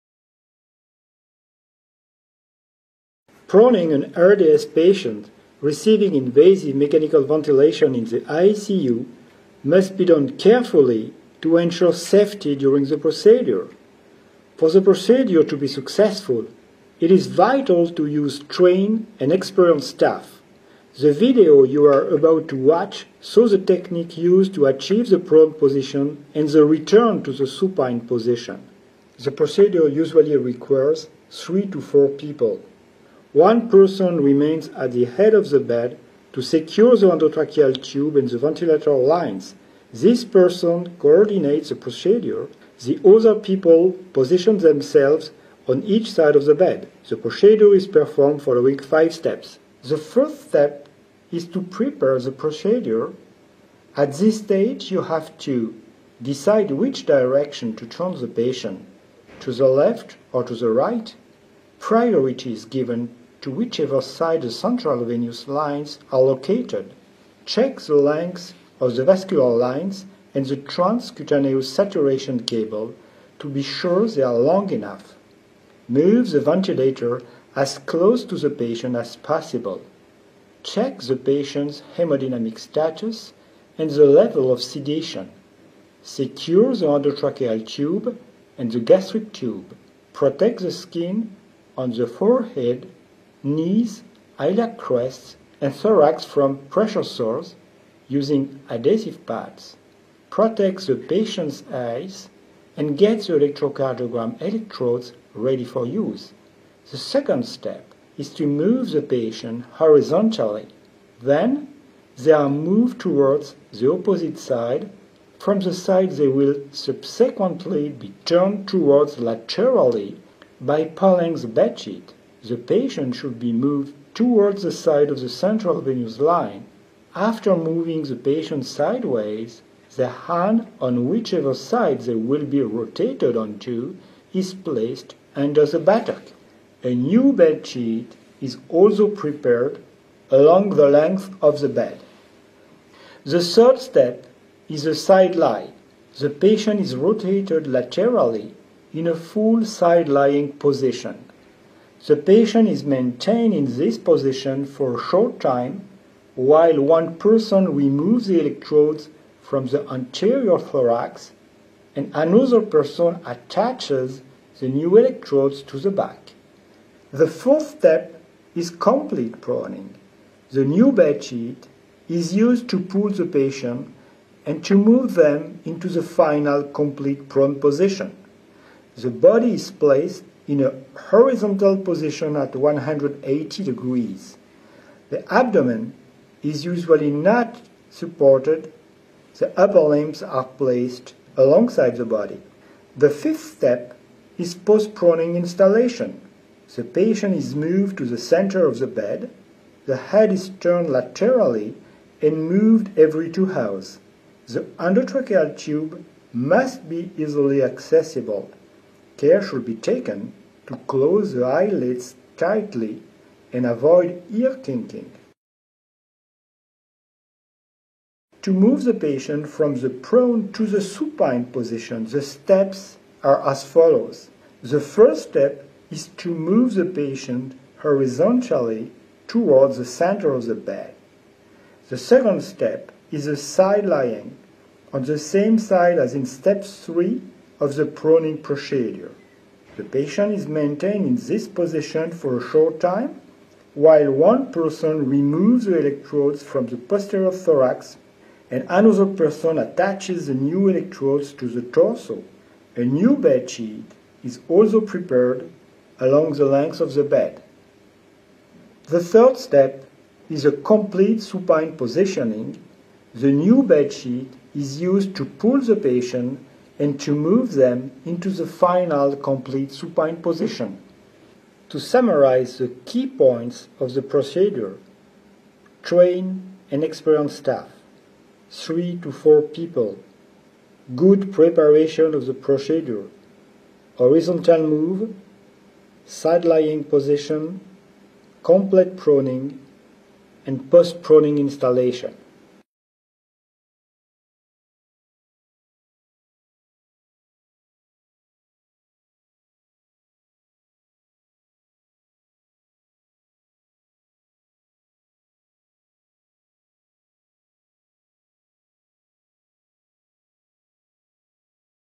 Técnica para llevar a cabo posición prono en SIRA( síndrome de insuficiencia respiratoria aguda )
El síndrome de insuficiencia respiratoria aguda (SIRA) es una causa importante de insuficiencia respiratoria aguda que se asocia a menudo con falla orgánica múltiple. Se caracteriza por la presencia de edema pulmonar e hipoxemia refractaria.